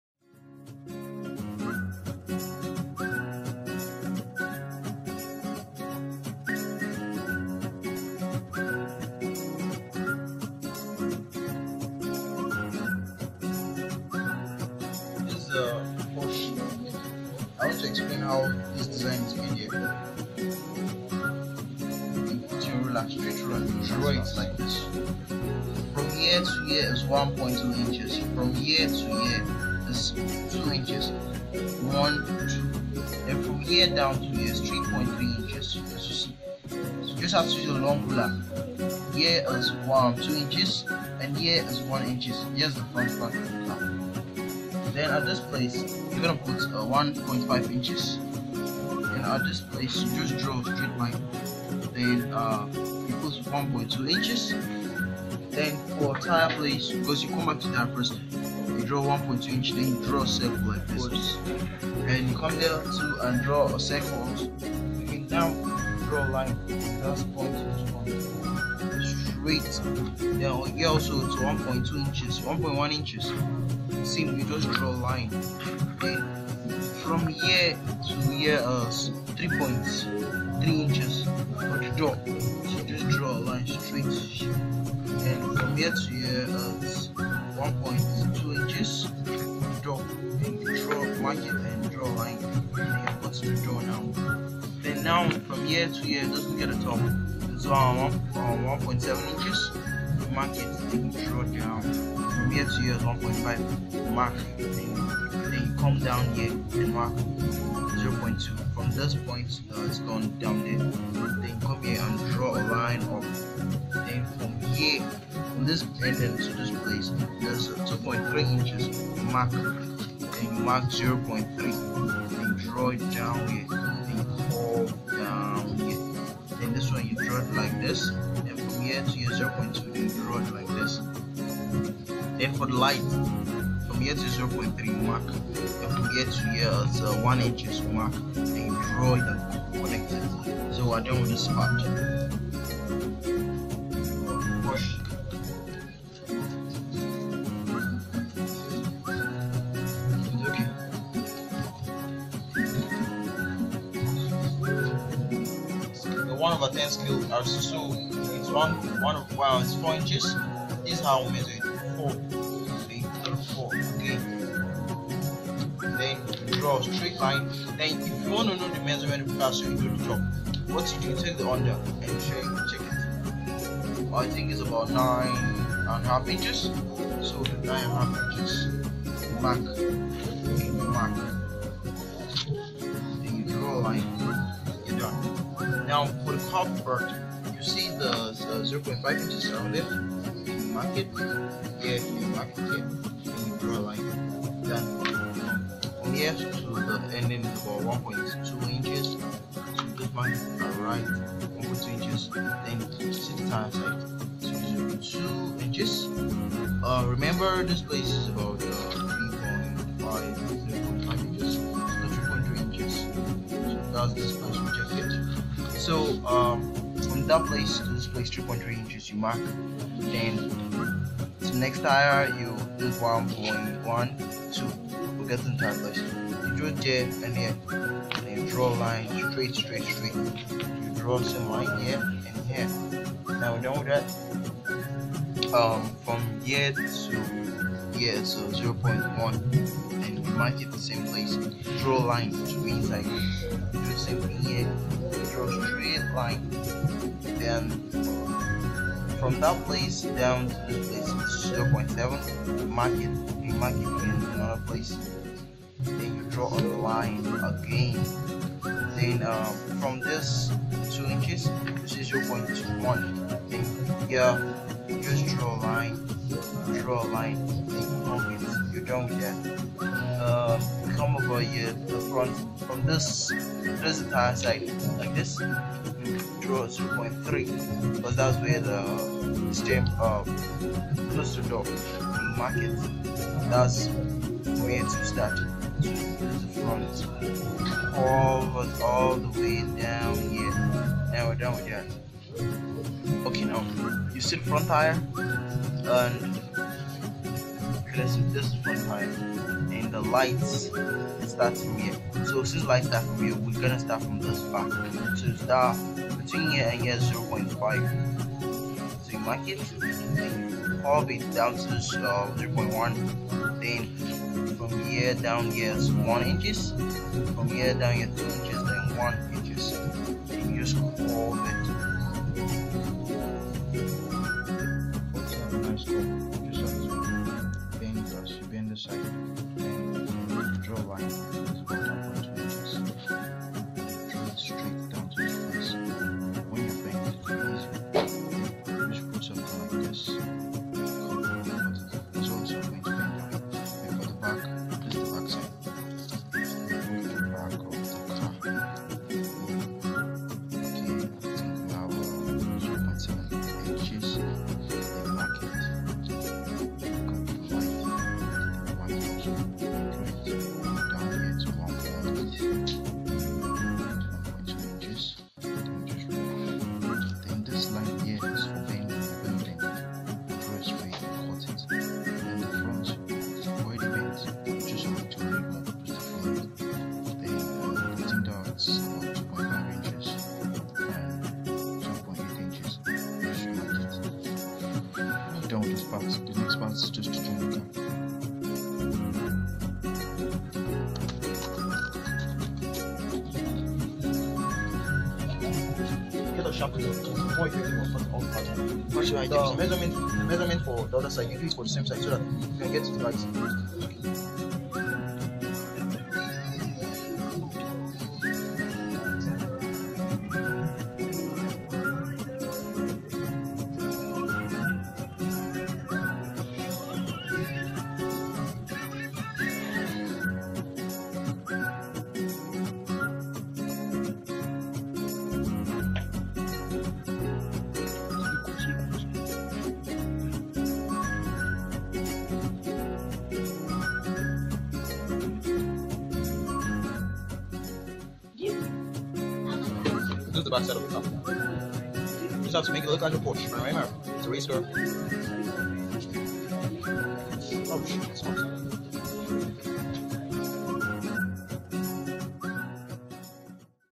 DIY ;How to Build Porche Car Body Explained PART 1
Build the Best car body with Emmy Pencil
Requirements :
• Photo of Porsche
• Drawing paper and pencil (optional)
•Cardboard
•Pen / Ruler / Scissors / Jack Knife
• spray or color
Procedure :
° get the picture of what you wanna do
° get your ruler and pencil
The procedure will. Be in WhatsApp chat.